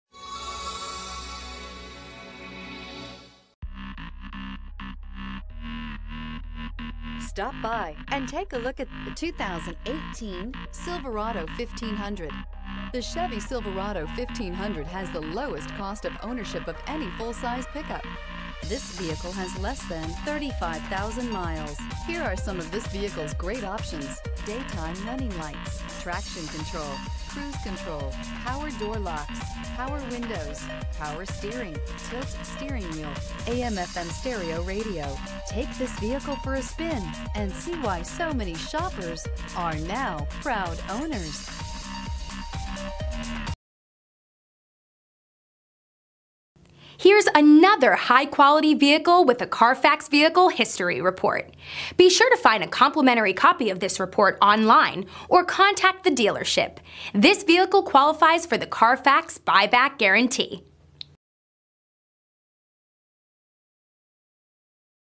2018 Chevrolet Silverado 1500 P4365
2018 Chevrolet Silverado 1500 Work Truck

617 N. Main St.

Summit White 2018 Chevrolet Silverado 1500 WT 4WD 6-Speed Automatic Electronic with Overdrive EcoTec3 5.3L V8 6-Speed Automatic Electronic with Overdrive, 4WD, Dark Ash With Jet Black Interior Accents Vinyl.brbrRecent Arrival! Odometer is 88009 miles below market average!brbrAwards:br  * 2018 KBB.com 10 Most Awarded Brands